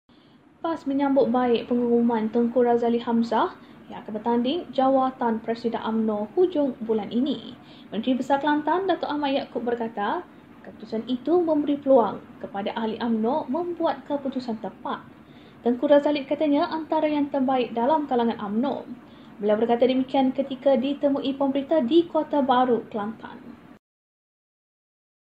Ku Li aka Tengku Razaleigh Hamzah Beri Peluang UMNO Pilih Yang Terbaik
PAS menyambut baik pengumuman Tengku Razaleigh Hamzah yang akan bertanding jawatan Presiden UMNO, hujung bulan ini.

Menteri Besar Kelantan, Datuk Ahmad Yakob berkata, keputusan itu memberi peluang kepada ahli UMNO membuat keputusan tepat.

Tengku Razaleigh katanya, antara yang terbaik dalam kalangan UMNO.